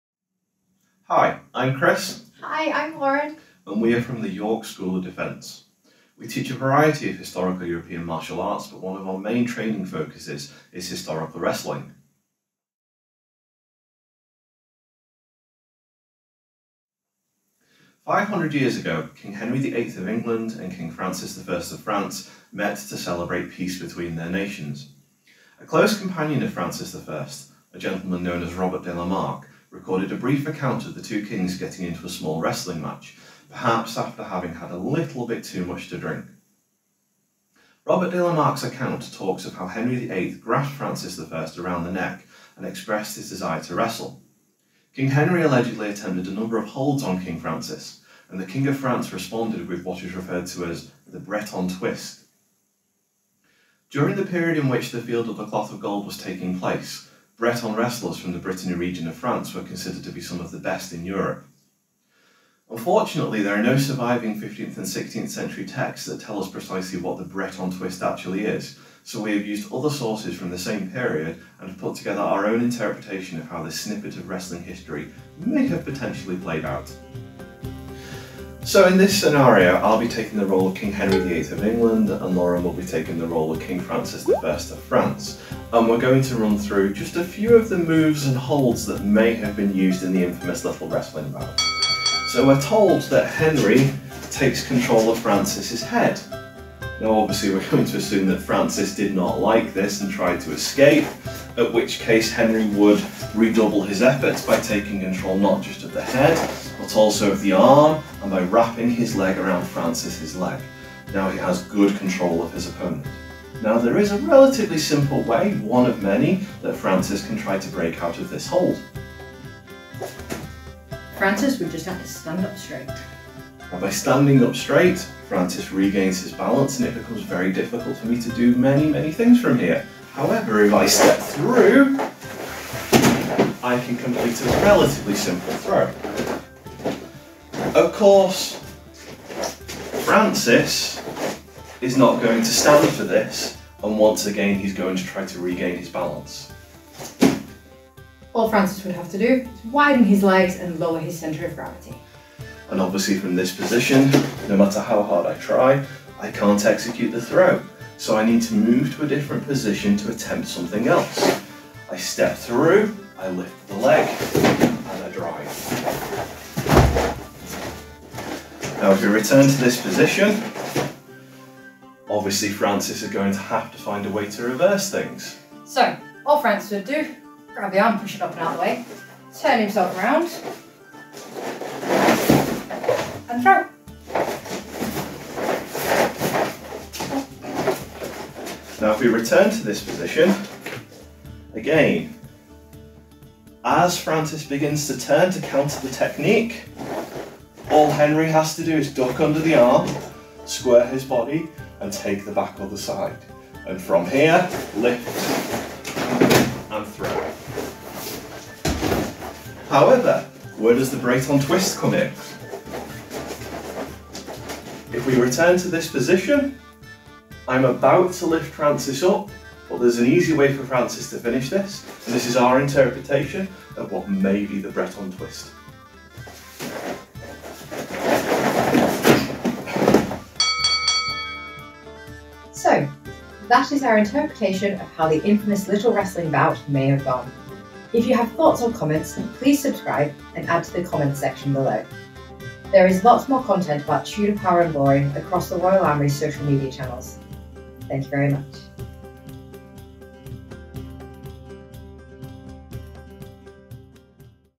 Wrestling with History
Chris and Lauren from the York School of Defence demonstrate how the wrestling match between Henry VII and Francis I at the Field of Cloth of Gold in 1520 may have played out based on their understanding of contemporary and other historical texts.

The Field of Cloth of Gold was a diplomatic summit between the Henry VIII of England and Francis I of France between the 7th - 24th June 1520. The summit was an extravagant display of royal magnificence which was expected to consolidate the peace between England and France after decades of warfare.

The event was a grand tournament involving jousting, tourneys, foot combat and of course the famous wrestling match between Henry VIII and Francis I. Immortalised in works of art and eye-witness accounts The Field of Cloth of Gold was one of the most opulent diplomatic and sporting events ever staged.